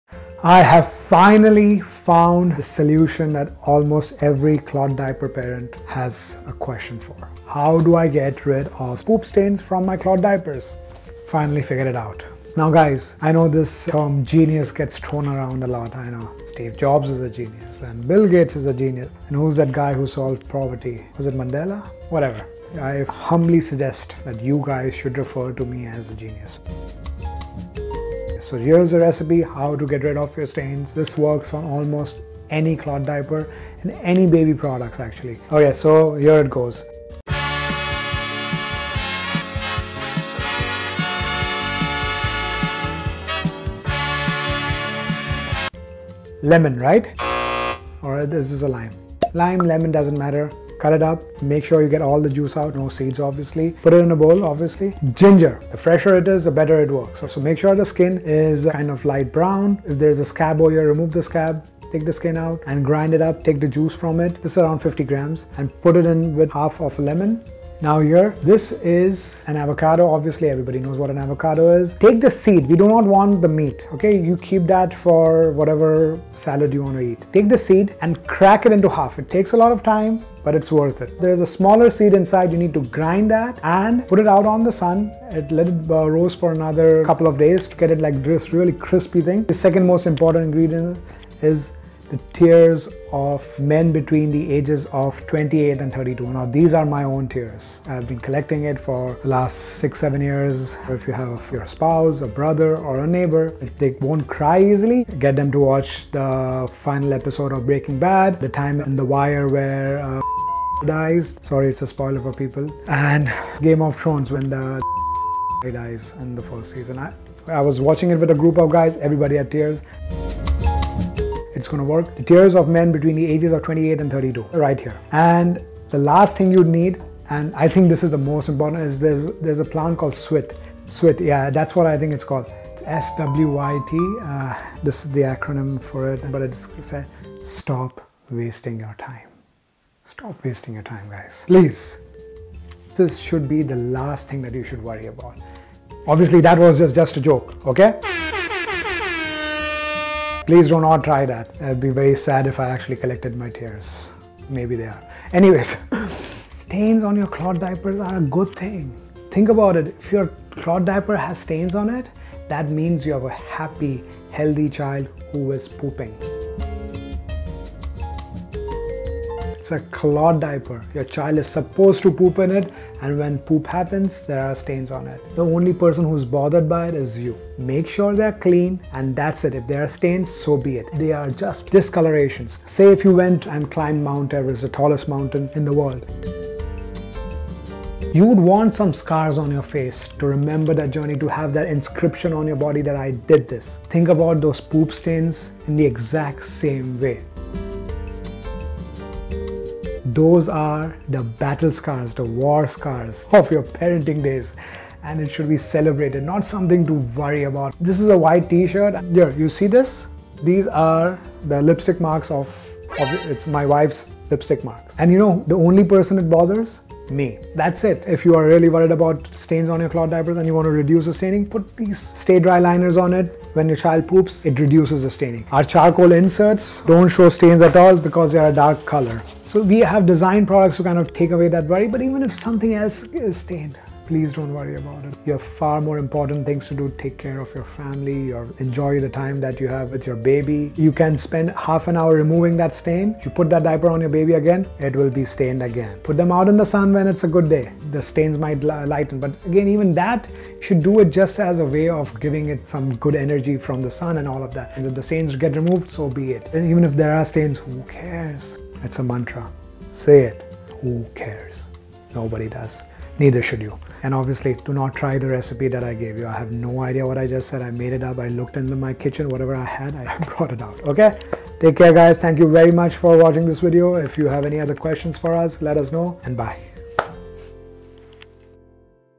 The CURE for Cloth Diaper Stains!
Today we are talking about one of the things disliked by cloth diaper users. poop stains.

One of the great things about our charcoal inserts is that they won't stain since they are dark! Unfortunately, our bamboo inserts and diaper covers can be stained. But we have great news! You can get rid of those stains if you really want to and without using a single chemical.

Ready for the answer? No, it's not some difficult recipe that Mohammed discovered *ahem, made up*.

The best way to get rid of poop stains is line drying your diapers. That's right, the SUN will remove the stains for you for free and without you doing any extra work.

But as Mohammed said, if you get rid of the poop stains, they will happen again next time your baby goes, so why worry about them? There are better things to do with your time instead of fretting over stains on something only you will see when changing your baby.

Here are a few things you can do instead-
*Play a game of peek-a-boo
*Make a cup of tea (like the ones we send to you with your order!)
*Eat some chocolate (same as the tea!)
*Watch an episode of your favorite show
*Take a nap

What would you rather do than worry about poop stains?